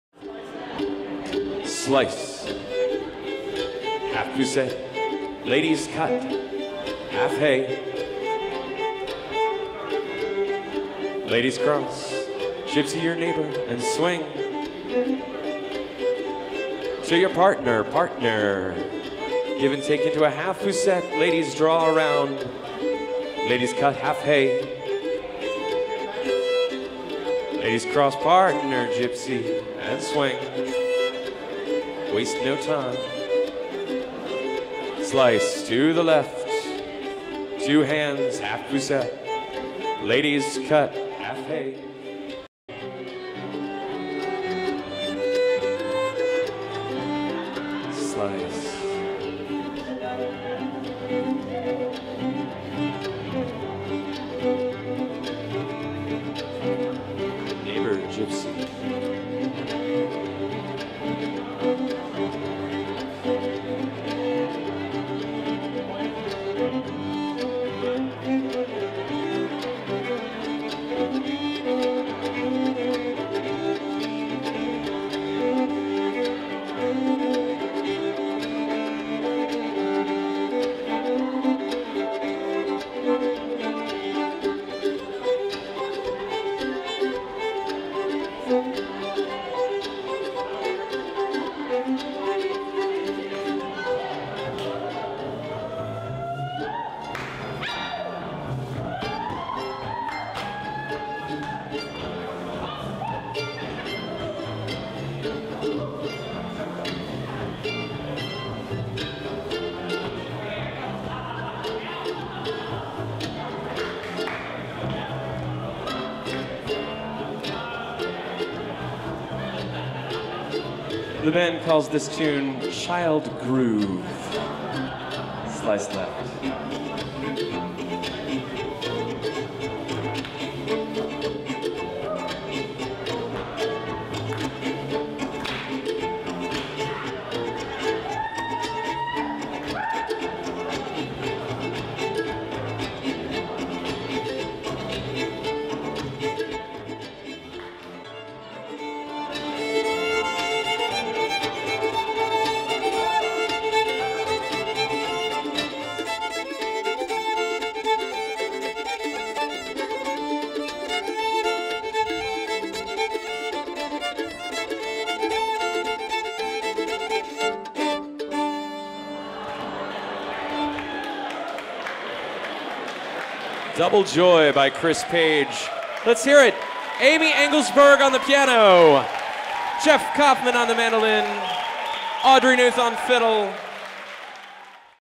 Catapult Showcase 2013 - Free Raisins & Tavi Merrill (loft view)
Catapult 2013.  Contra Dancing on Saturday (5/25/13) at Catapult featuring music by Free Raisins and calling by Tavi Merrill. "Double Joy" by Chris Page.

Contra Dance at Catapult!